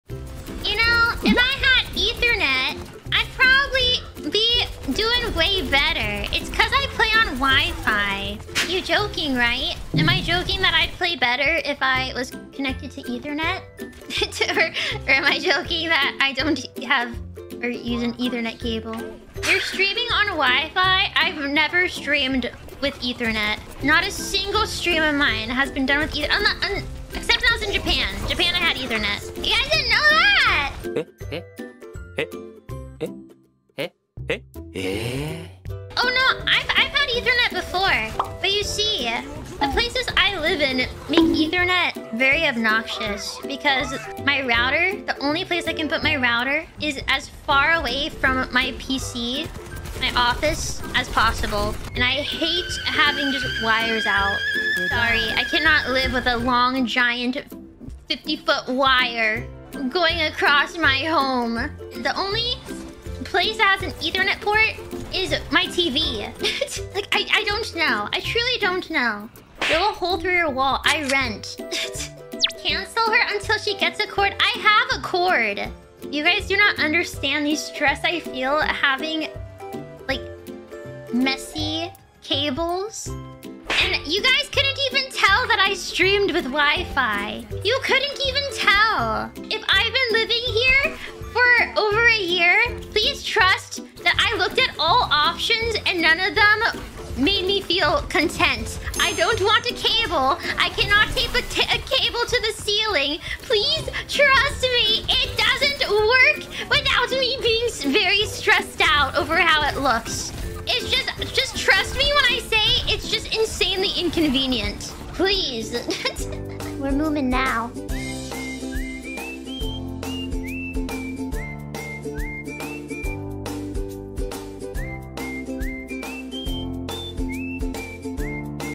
[ENG SUB/Hololive] Mumei has been streaming using WiFi only for the past year
Credits :
BGM